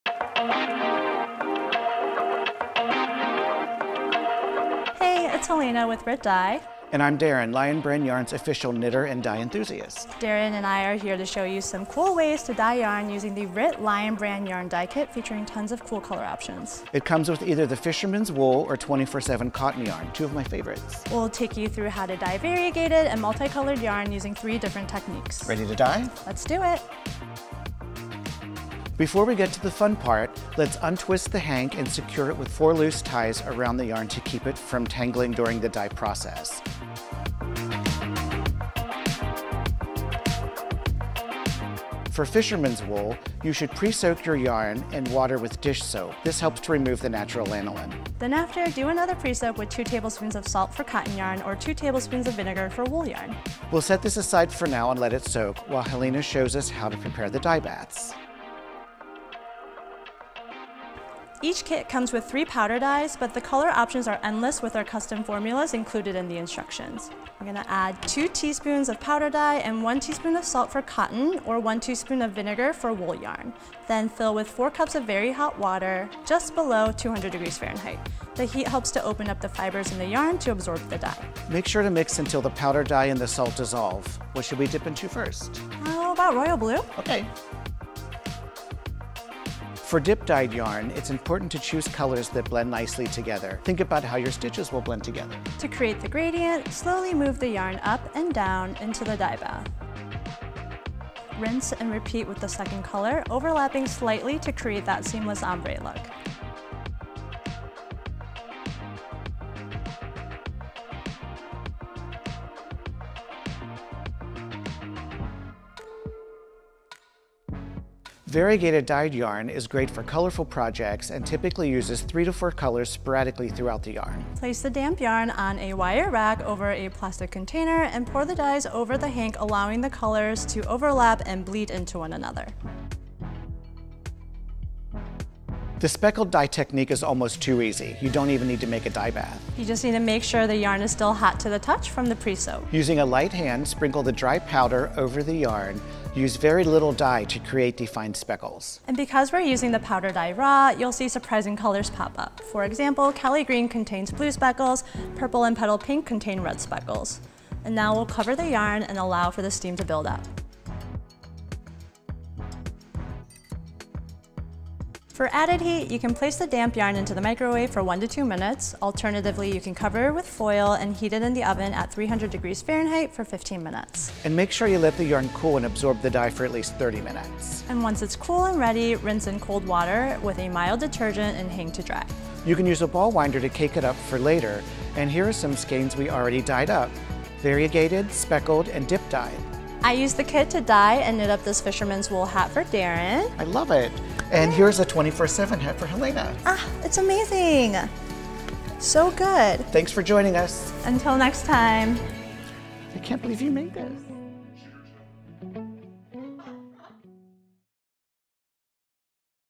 Rit x Lion Brand Yarn Dye Kits
Explore the art of yarn dyeing and enter into a world of color with Rit® and Lion Brand® Yarn Dye Kits. We've chosen two perfect yarns for dying, Fishermen's Wool®, a 100% virgin worsted weight wool, and 24/7 Cotton®, a worsted 100% cotton, with well-known and loved Rit® dyes. Each DIY kit includes the one yarn hank, 3 powder dyes, and instructions to get you started! One kit can dye up to 10 hanks of yarn.